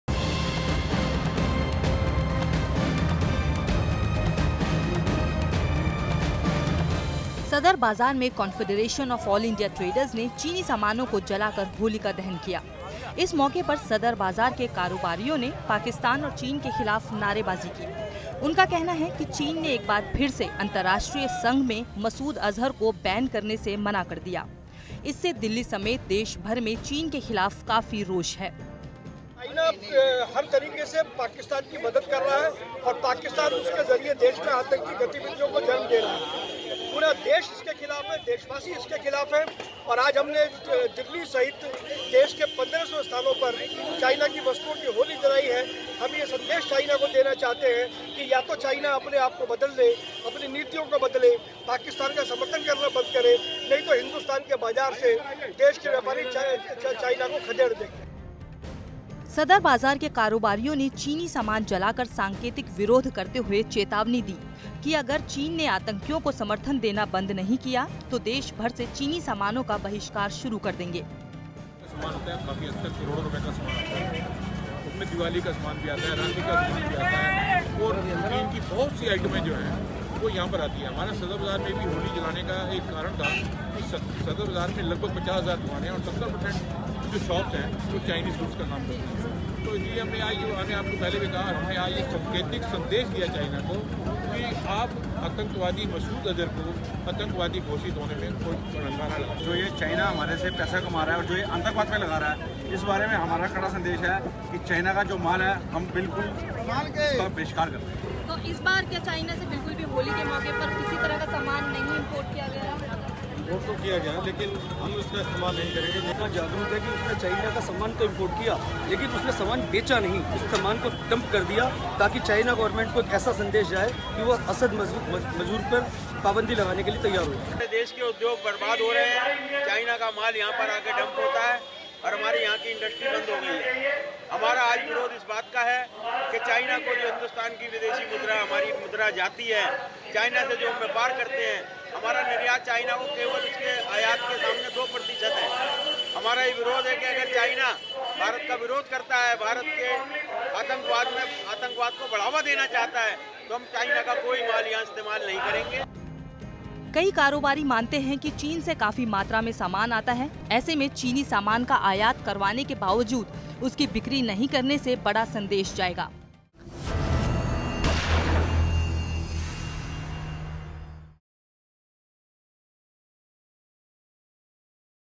होली पर भारत ने चीन को सिखाया कुछ ऐसे सबक| Crime Tak
सदर बाजार में कॉनफीडरेशन आफ आल इण्डिया ट्रेर्डस ने आज चीनी समानों को जलाकर होलिका दहन किया. दरअसल चाइना के एक बार फिर से अन्तराष्ट्रीय संघ में मसूद अज़हर को बैन करने से मना कर दिया और अपना पैर पीछे खींच लिया. जिसके बाद दिल्ली ही नहीं पूरे देश में चाइना के ख़िलाफ़ बहुत रोष नजर आ रहा है..